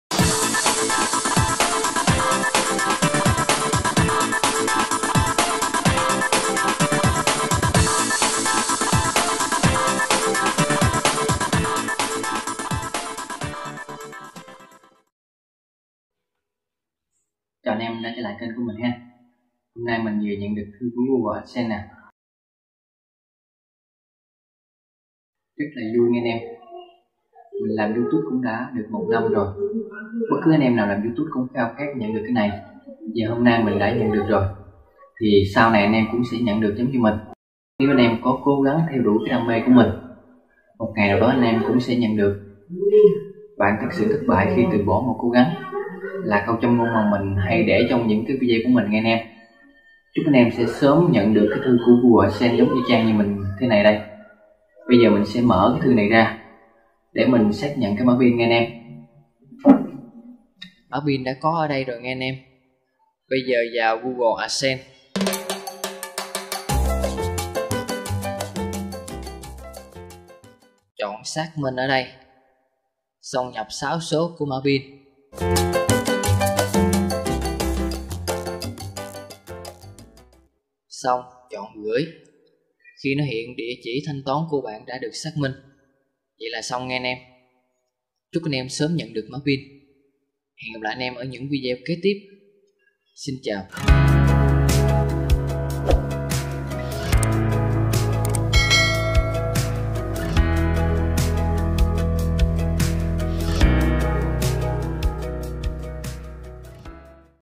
Hướng dẫn xác minh mã pin | Phạm Tâm vlogss
Hướng dẫn xác minh mã pin
Thân chào anh em đã đến với kênh Phạm Tâm Vlogss
Trong video này mình chia sẻ cho anh em xác minh mã pin khi nhận được thư của google adsense. Niềm vui lớn nhất khi làm youtube là nhận được thư của google adsense chúc anh em sớm nhận được mã pin giống mình để có thể nhận những khoản thanh toán từ YouTube.